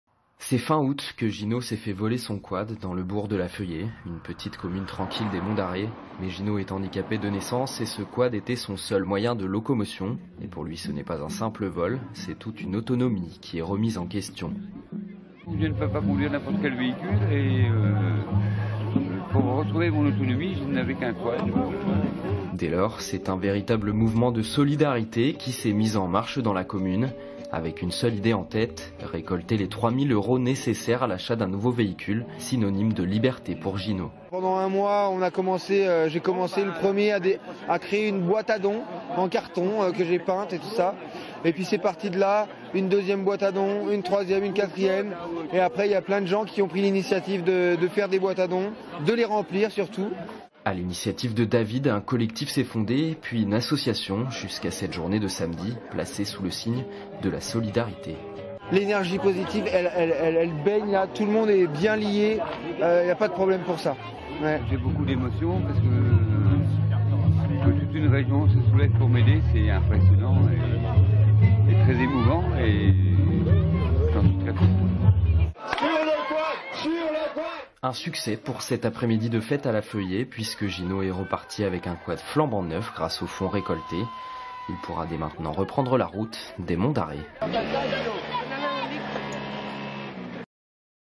Un Quad pour Gino : Une journée de solidarité à La Feuillée
Sites webs/blogs, copiez cette vidéo avec le bouton "Intégrer" !
Les habitants de La Feuillée dans le Finistère se sont mobilisés pour offrir un nouveau Quad à Gino, un handicapé moteur. Pour exploitation TV, droits réservés, nous contacter